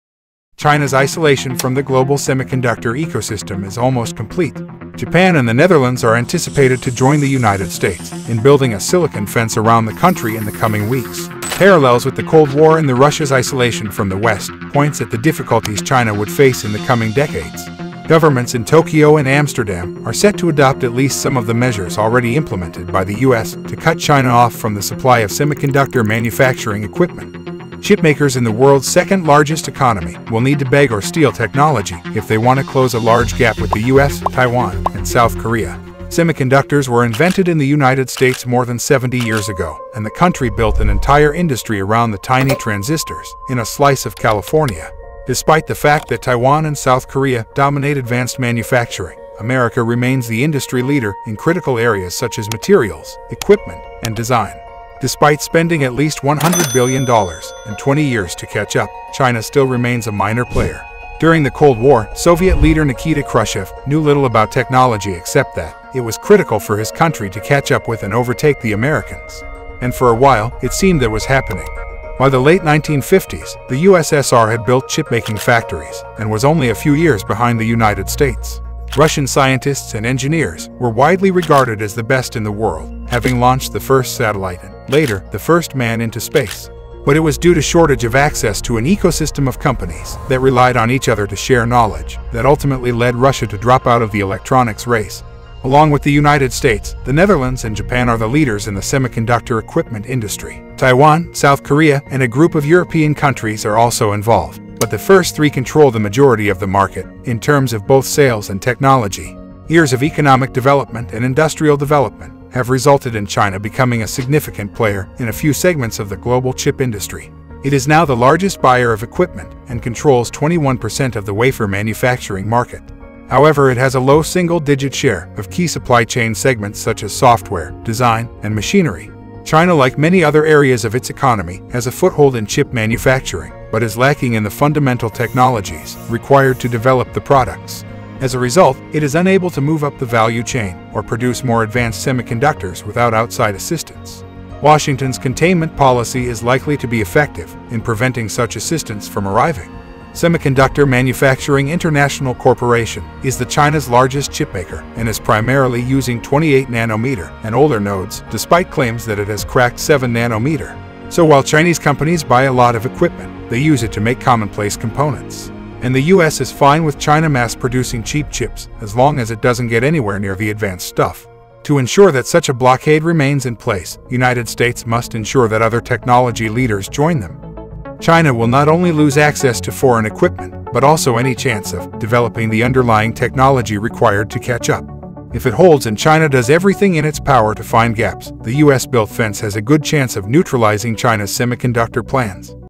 How China’s Isolation from the Semiconductor Ecosystem is Almost Complete
China is the world’s largest semiconductor market, and its appetite for chips has been on an unbroken growth spurt for years. As recently as five years ago, Korea and Taiwan together supplied more than half the chips used in China. Now they supply less than a tenth and is at the verge of Suffocation.

Video Courtesy: WION, Asianometry, DBS.